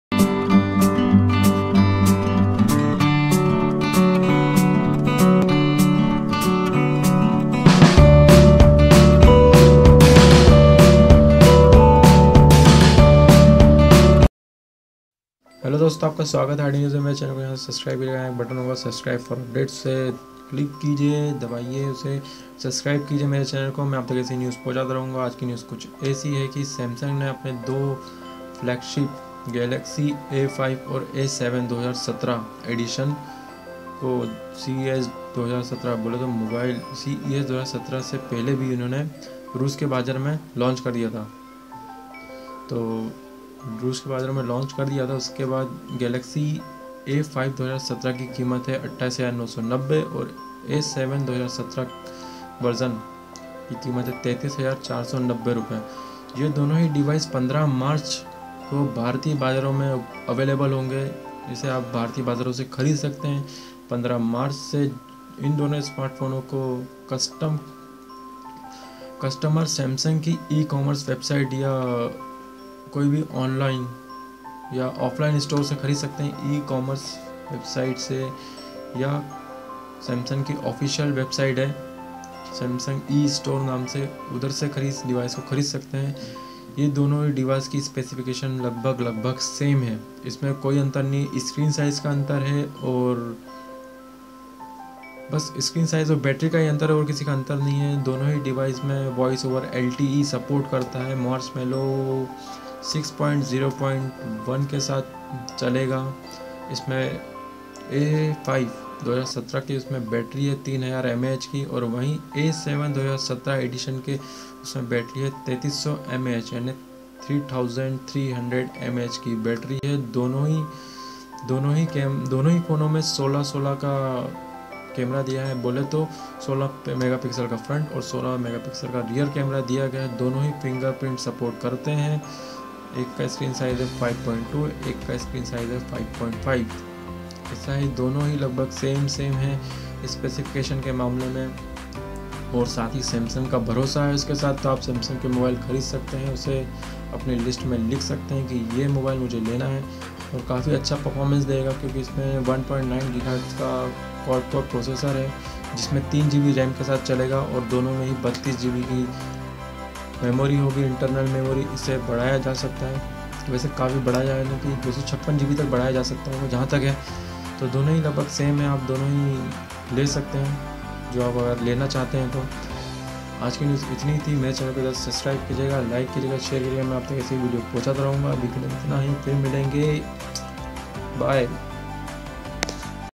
सैमसंग ने लॉन्च किया गैलेक्सी A5 (2017) और गैलेक्सी A7 (2017), जानें कीमत और स्पेसिफिकेशन
-~-~~-~~~-~~-~- autoAndTech Autonewswwithrahul A&T A&TWithRahul TechNewsWithRahul #AutoAndTechWithRahul rahuldeshmukh Aam Hindustani Ka Review